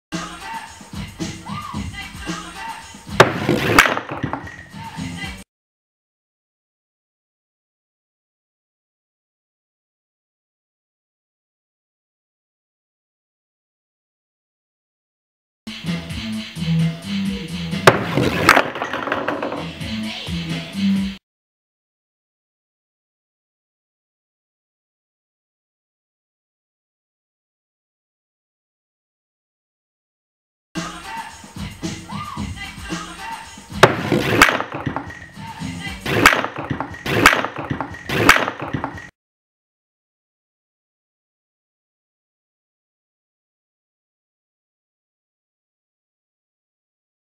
Strikes on the mini lane
Just a short video of a few strikes on my half scale mini bowling lane